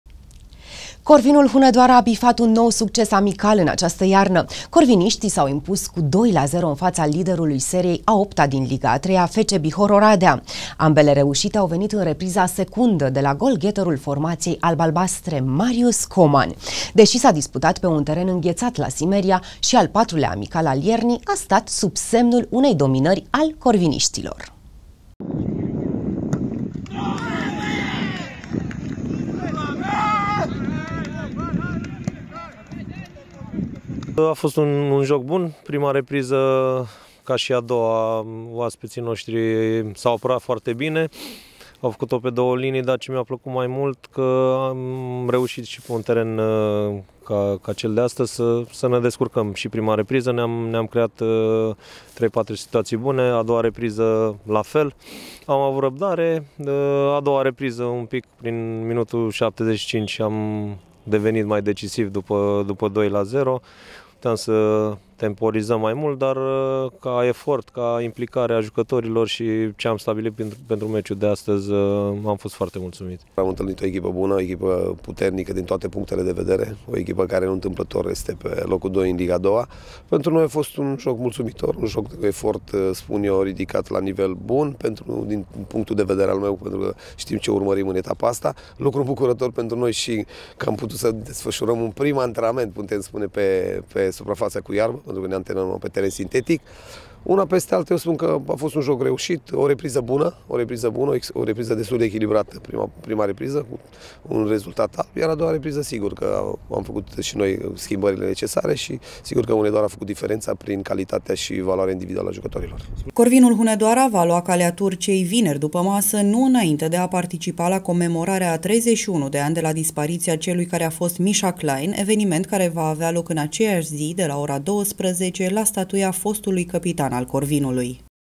CORVINUL ÎNCHEIE PRIMA SERIE DE AMICALE CU SUCCES
ECHIPA ALB-ALBASTRĂ PLEACĂ, VINERI, ÎN CANTONAMENT ÎN TURCIA
             Corvinul Hunedoara a bifat un nou succes amical în această iarnă. Corviniștii s-au impus cu 2-0 în fața liderului seriei a opta din Liga a treia, FC Bihor Oradea. Ambele reușite au venit în repriza secundă de la golgheterul formației alb-albastre, Marius Coman. Deși s-a disputat pe un teren înghețat la Simeria, și al patrulea amical al iernii a stat sub semnul unei dominări al corviniștilor.